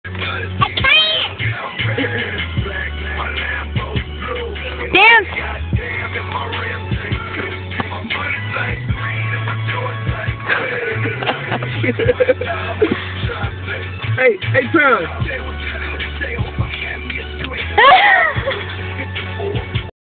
trey and wes
little 3 yr old trey and wes in my car lol